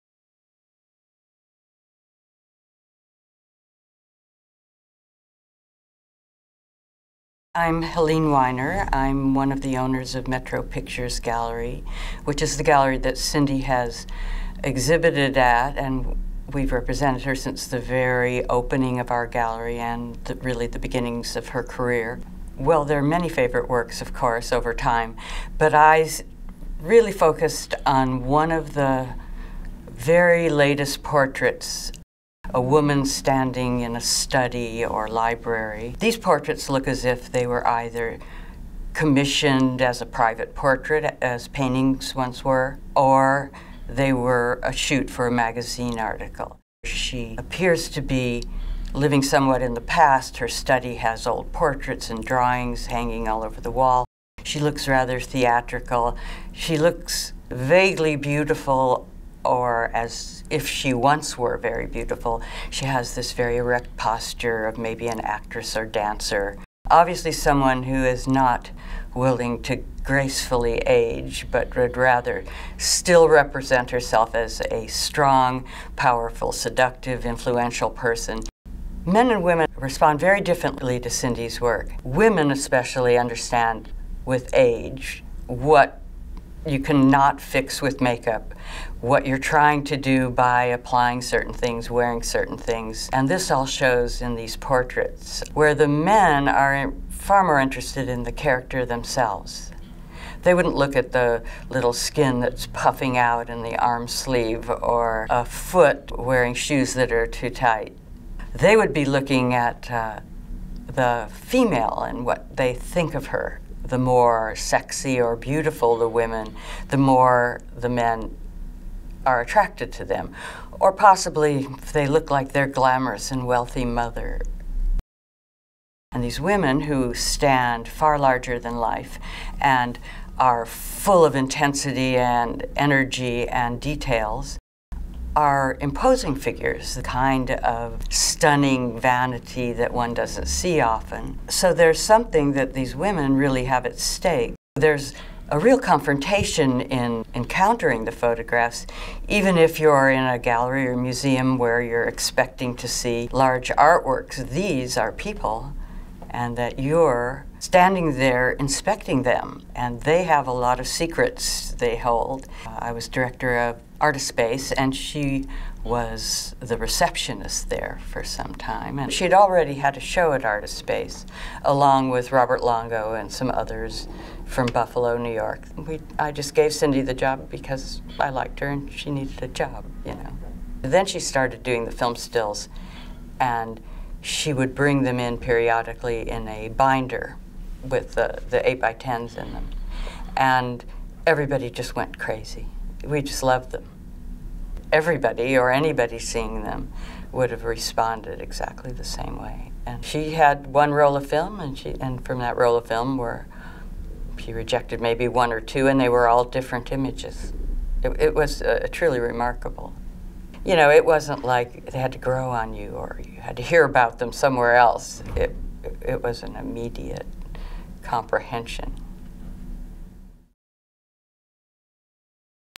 Helene Winer on Cindy Sherman's Untitled #474, 2008
Metro Pictures gallery owner Helene Winer discusses Cindy Sherman's Untitled #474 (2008).

Part of the exhibition Cindy Sherman, on view at the Walker Art Center November 10, 2012 to February 17, 2013.

Filmed by David Shuff, Calvin Robertson, and Ben Coccio